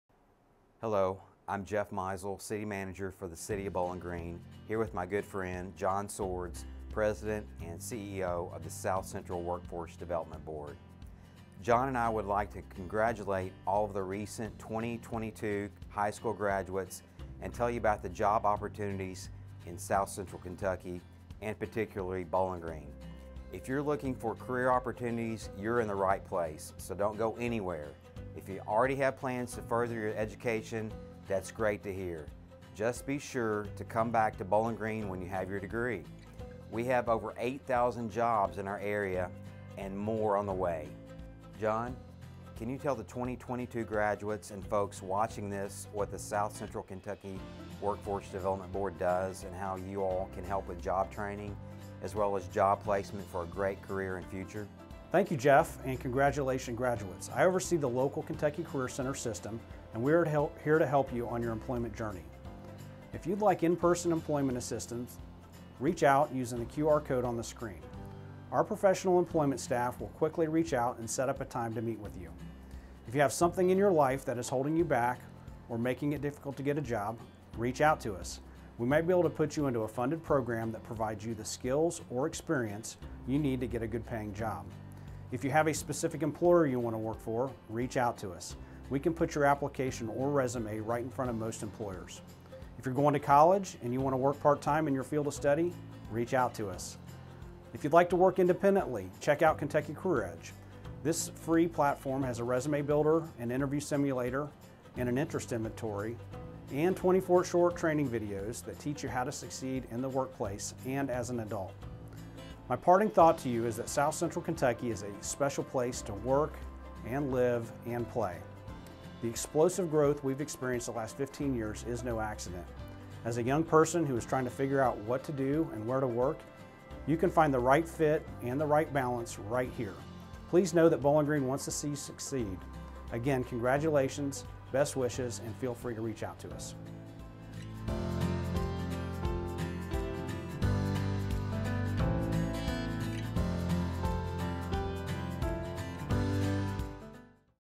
Grad Message
Congratulations high school graduates. We invite you to make Bowling Green a permanent chapter in your story. There are many job opportunities right here as well as a four-year college and a technical school.
South Central Workforce De